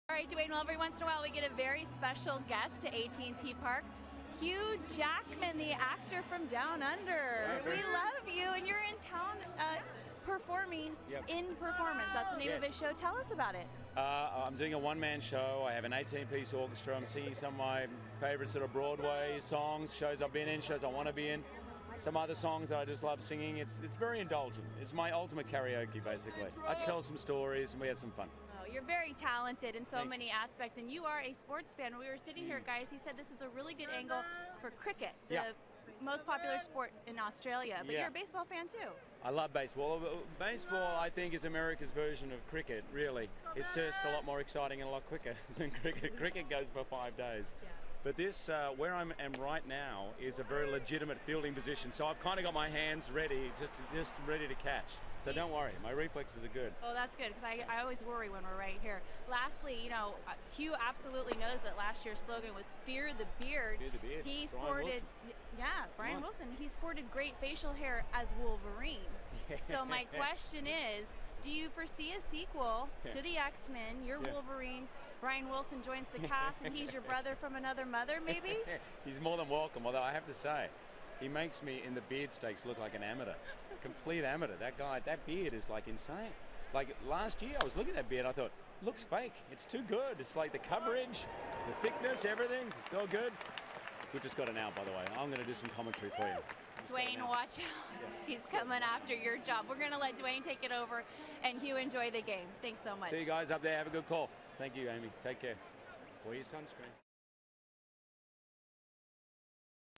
2011/05/12 Hugh Jackman at Giants game
ARI@SF: Actor Hugh Jackman takes in the Giants game